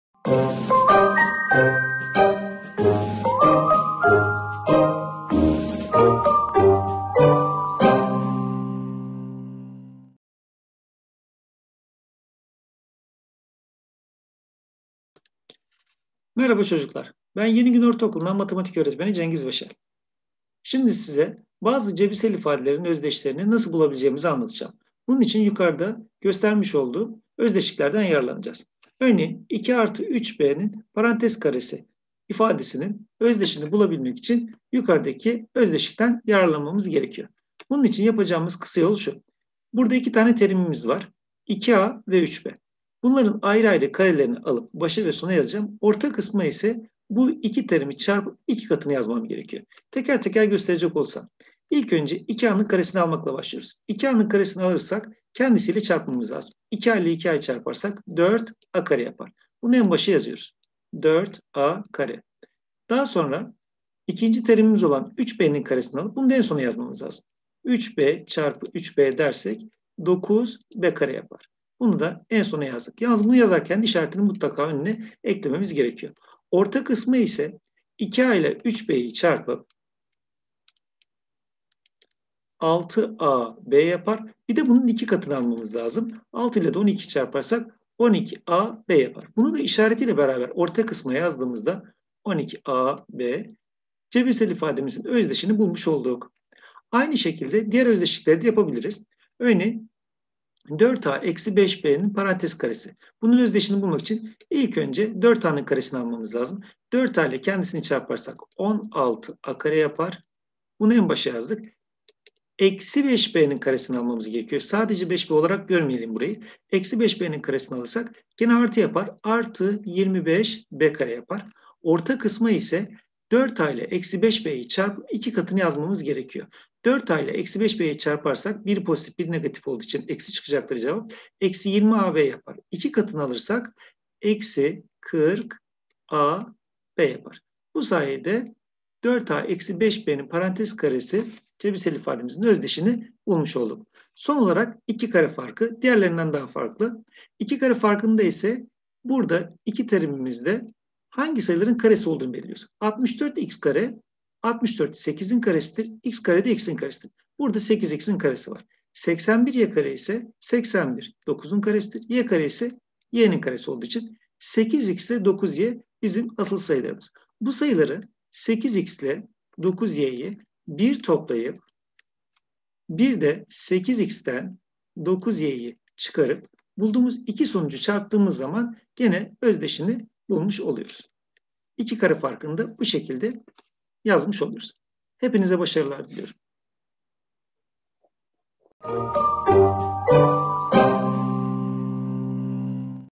Yenigün Ortaokulundan “LGS’de Matematik Dersi Püf Noktalar” videosu 3
Sevgili öğrenciler; LGS öncesi gözden kaçırdığınız püf noktaları vurgulamak amacıyla Adapazarı İlçe Milli Eğitim Müdürlüğüne bağlı okullarımızda görev yapan değerli öğretmenlerimizin hazırladığı videolar sizleri bekliyor.